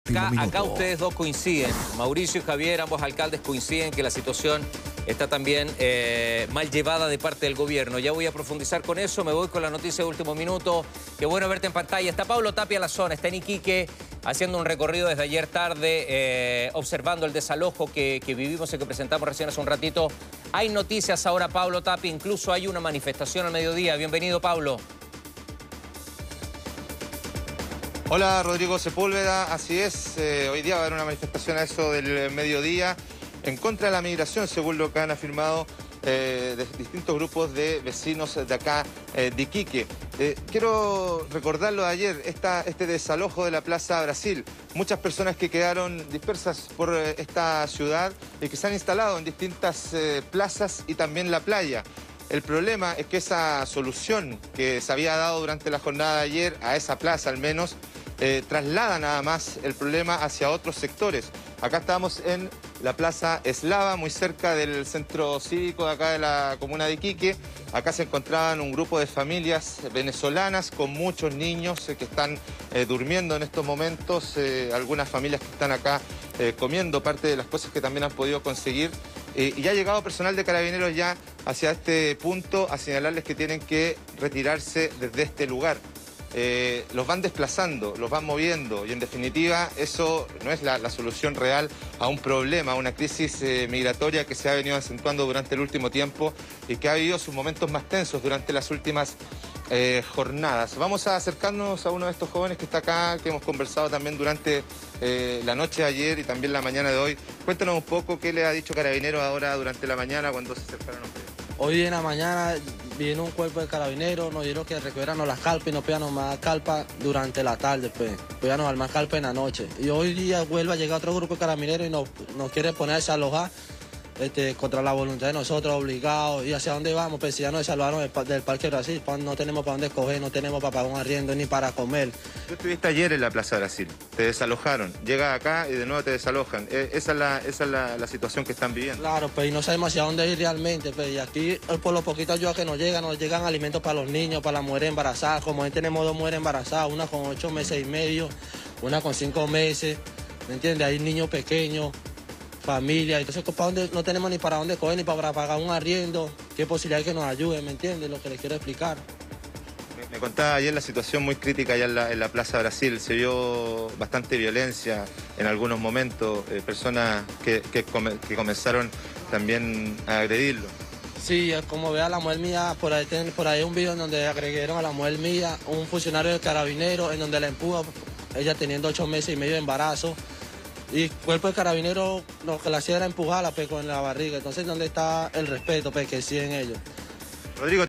El dramático relato de migrantes venezolanos desalojados en Iquique: "No sabemos hacia dónde ir"
Un grupo de migrantes venezolanos que fue desalojado este viernes desde la Plaza Brasil, de Iquique, reclama que "no sabemos hacia dónde ir", luego que pasaran la noche en el sector de la Plaza Eslava, y nuevamente Carabineros les solicitara retirarse de ese lugar.